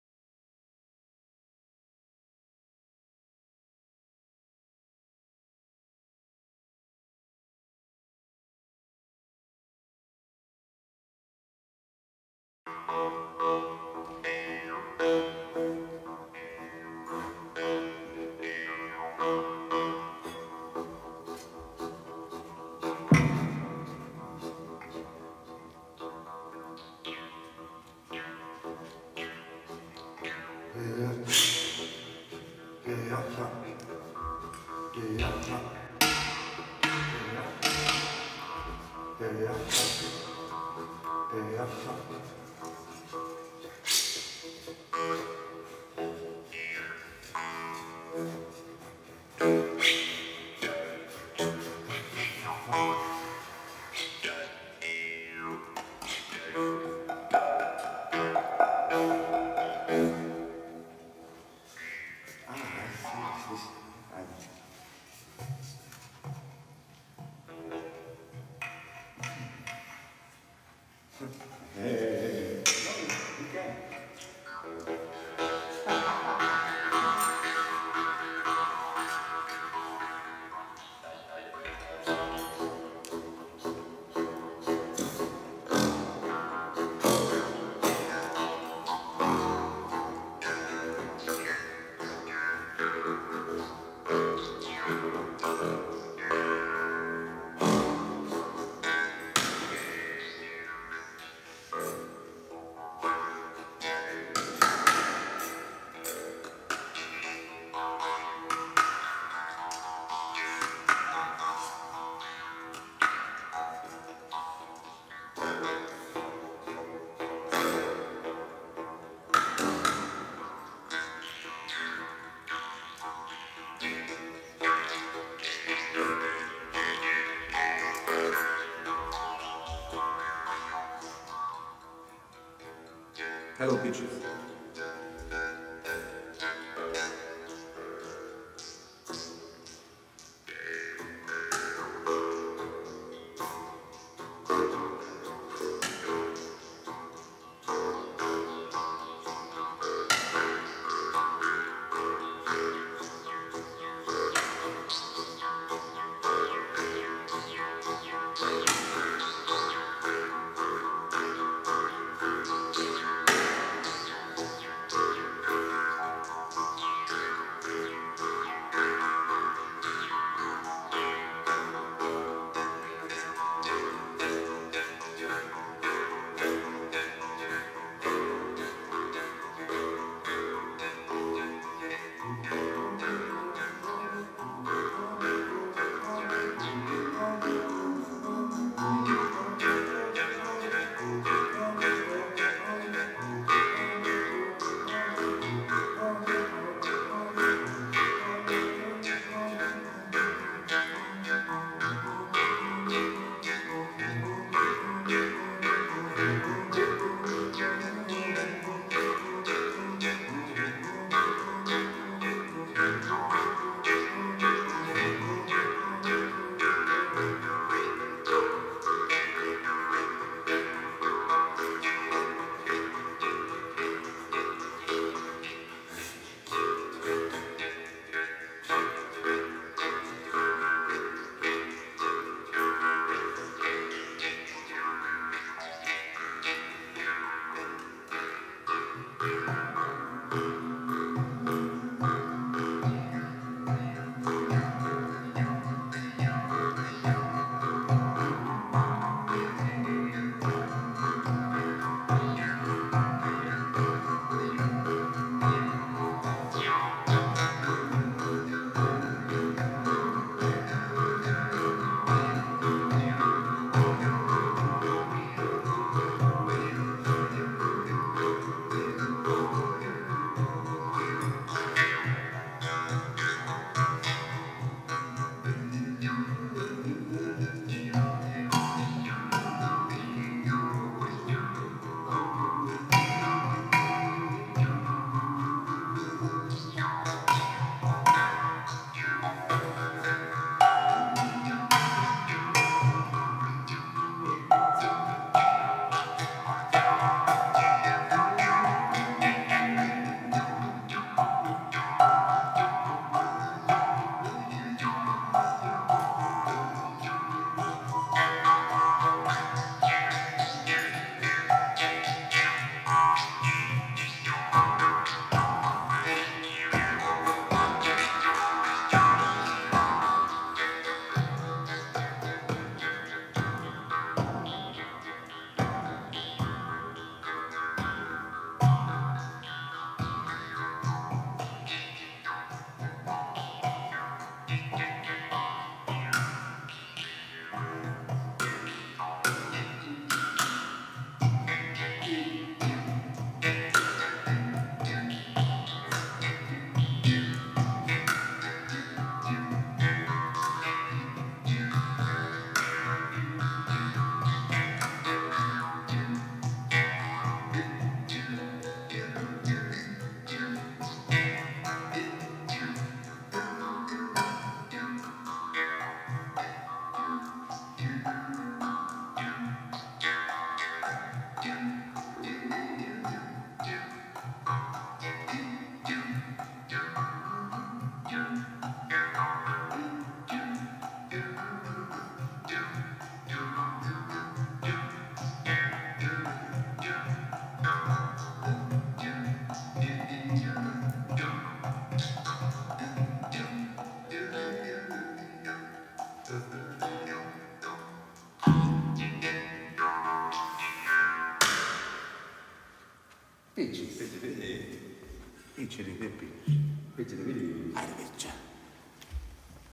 Jew's Harp improvisation, Doromb Camp 2019, part IV
Special improvisation with Hungarian water jugs (Ceglédi kanna).

Jew's Harp Trio: Pitchies @ the Church in Polgárdi, Hungary.
Pedro Acevedo from Asturias (Spain), Gula Bódis from Fejér (Hungary) and Patrick Frey from Bern (Switzerland). The 3 musicians got to know each other on the music platform Soundcloud (virtual) and realized very quickly that they share a common passion: the jew's harp. Despite their geographical distance, they occasionally meet to make music together. In June 2019 they met for 7 days at Gyula's home in Szabadbattyán.